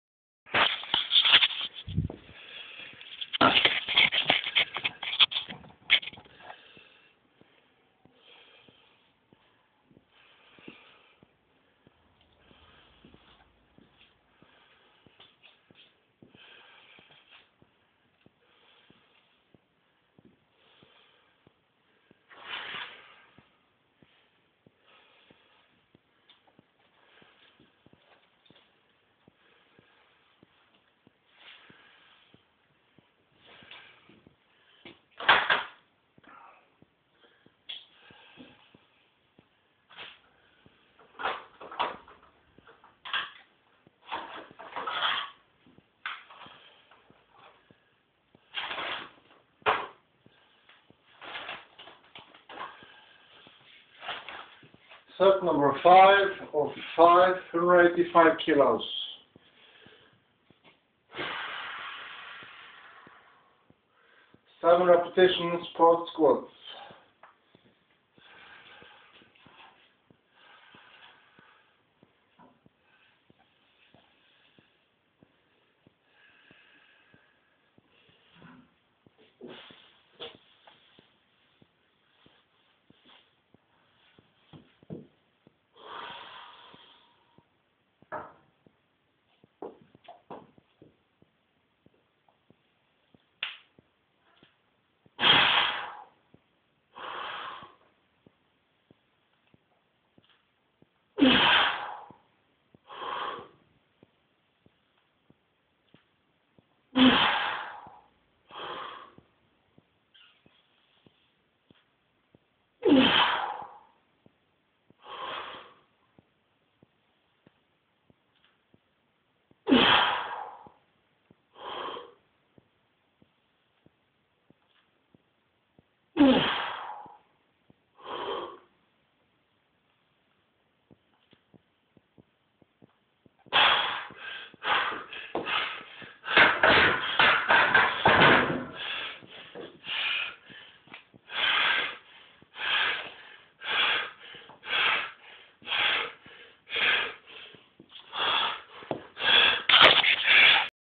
185kg x 7 (Road to 260kg squat)
Did 185kgx5x7 today. +2kg PR (Previous best was 183kgx5x7). It was set autumn 2009, and tied one week ago. Very motivating to have PR workouts like this. On this video, is the last set of todays workout, ie. set 5/5.